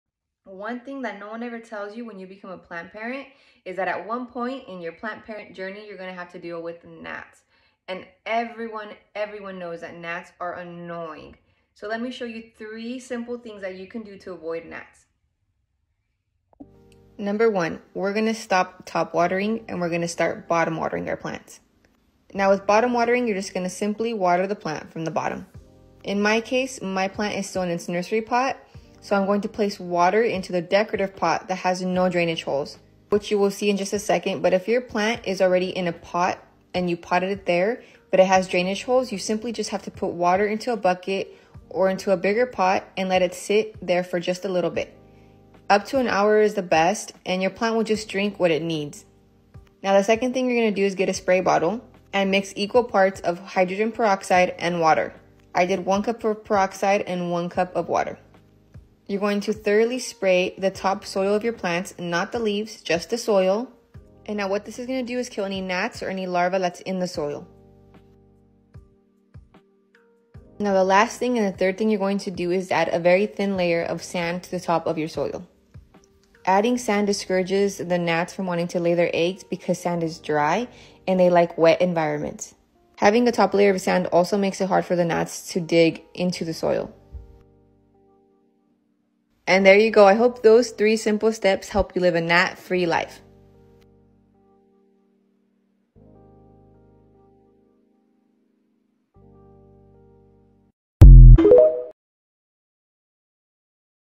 3 simple steps on how to avoid Fungus Gnats 🪴plantmom
@justgessc 3 simple ways to avoid Fungus Gnats
Theses 3 things have helped me keep them away from all my plants!

Hi Amigas I’m a plant señora who loves her plantitas! I hope theses tips help you out! If you have any for me please share I’m always up to learning new tips and tricks 🫶🏻🪴

And same for Instagram 🫶🏻
I will follow back let me know you came from YouTube!